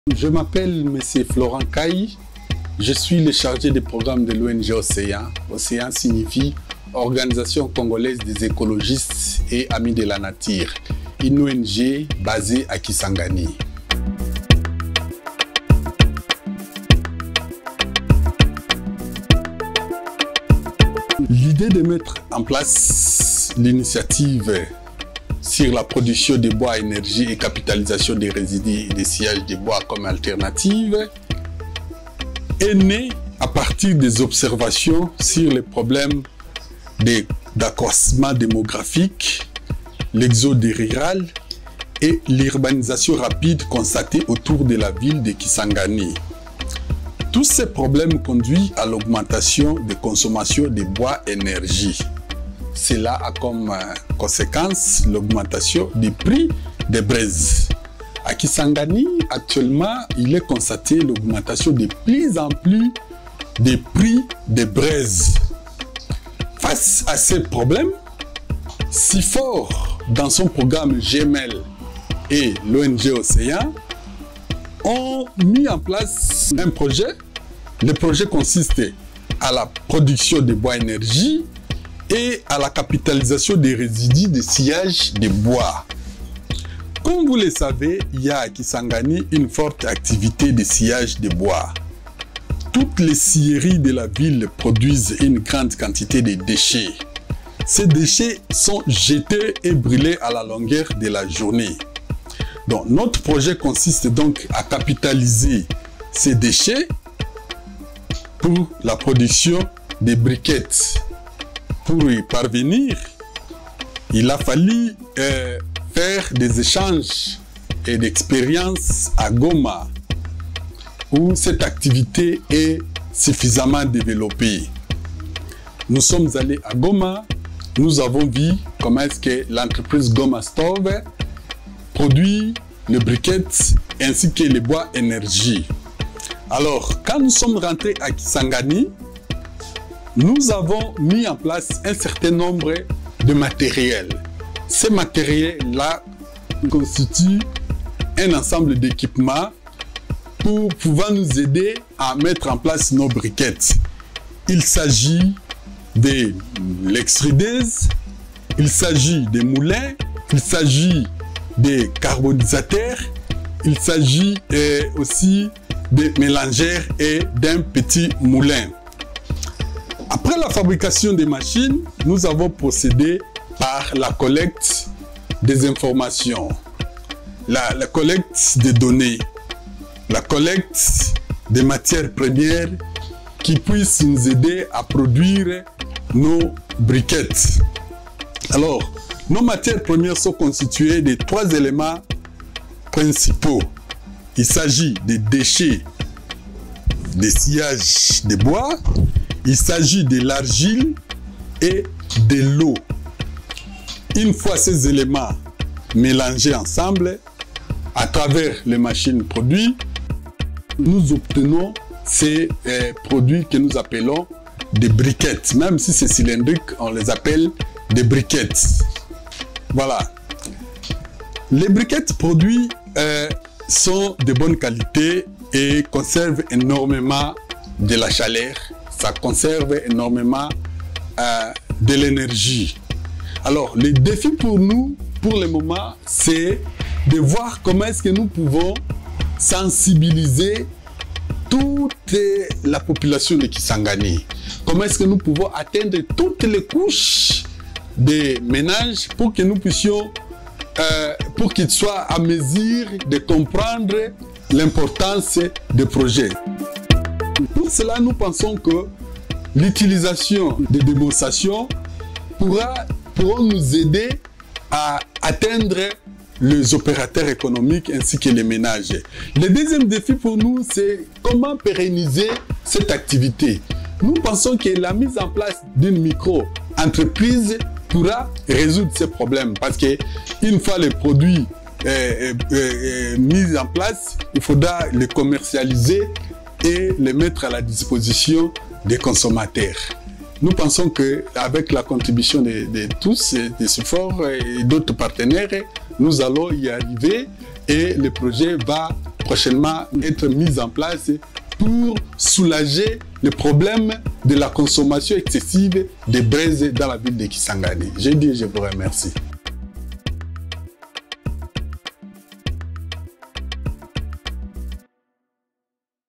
DRC: Producing charcoal briquettes from sawmill waste
The organization OCEAN is producing charcoal briquettes by using sawmill waste in Kisangani, Democratic Republic of the Congo. The briquettes offer the urban population a sustainable wood fuel alternative to cook their meals.